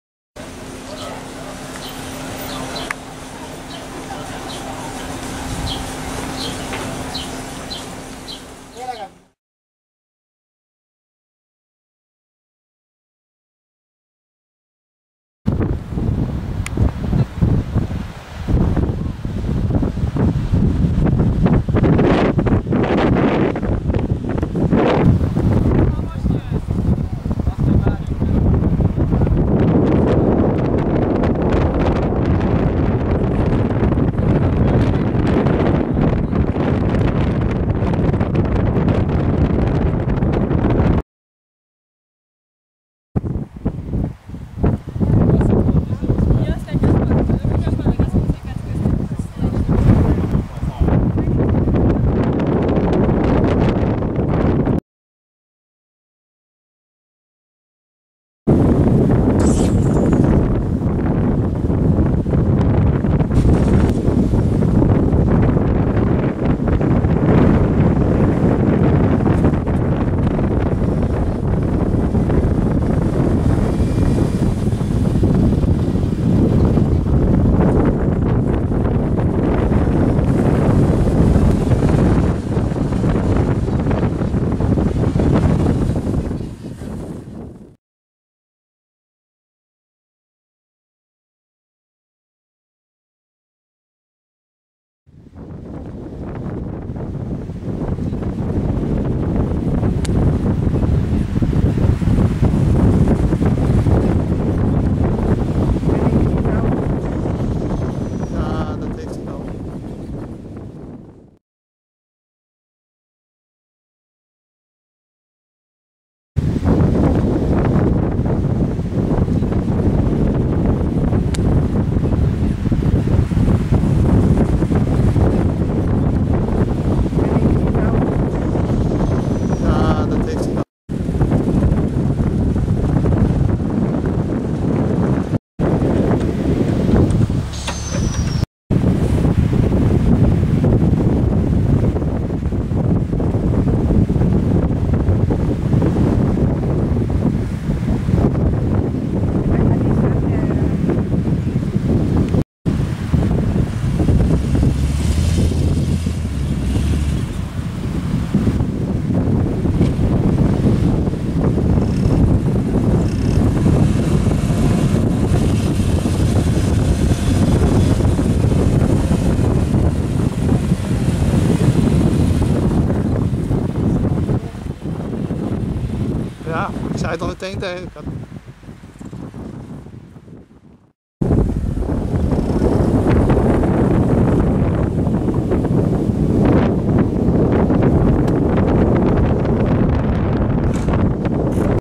prasonisi beach car stuck in the sand
Cars stuck on beach Rhodos.    Easy money for kitesurfclub